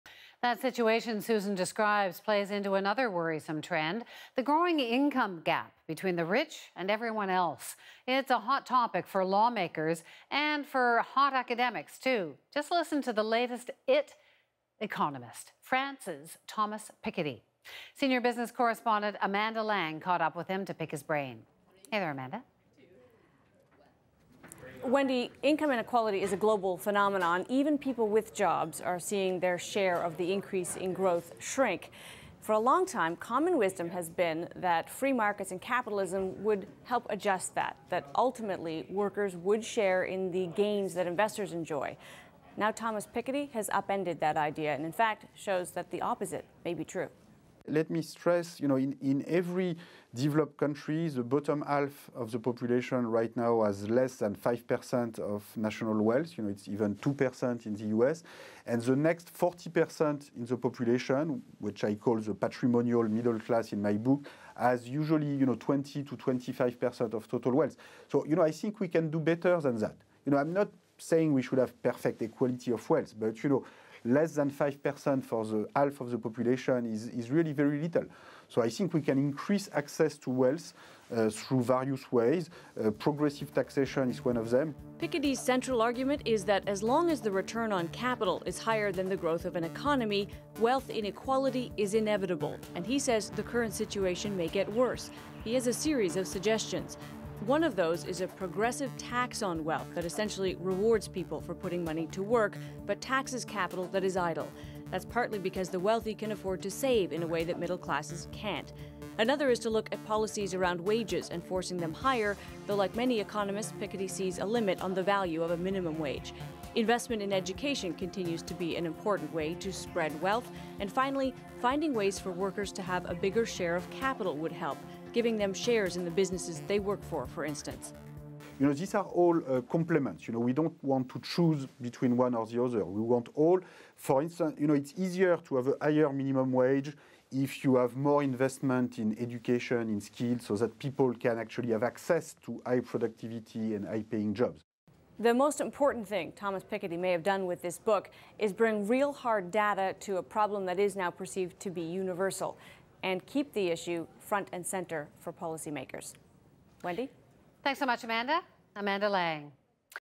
Thomas Piketty explains his views on inequality
The growing income gap between rich and poor is a hot topic right now. As a result, the writings of French economist Thomas Piketty on inequality have hit a chord.